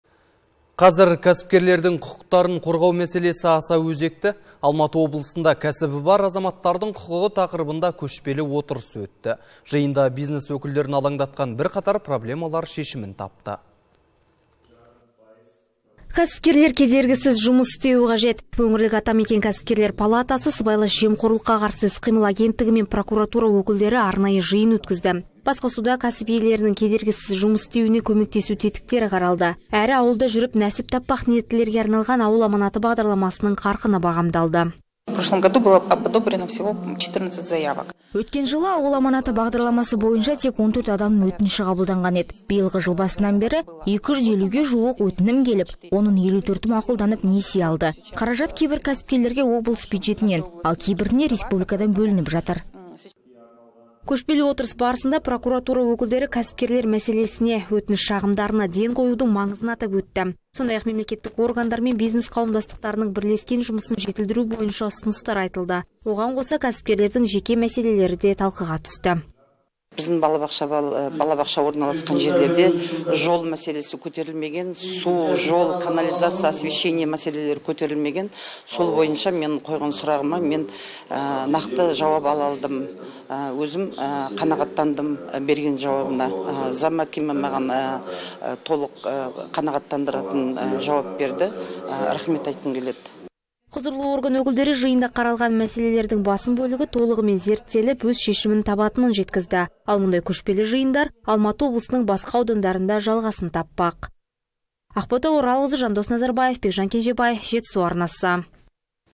Алматы облысында кәсібі бар азаматтардың құқығы тақырыбына арналған отырыс өтті
Қазір кәсіпкерлердің құқықтарын қорғау мәселесі аса өзекті. Алматы облысында кәсібі бар азаматтардың құқығы тақырыбында көшпелі отырыс өтті. Жиында бизнес өкілдерін алаңдатқан бірақатар проблемалар шешімін тапты.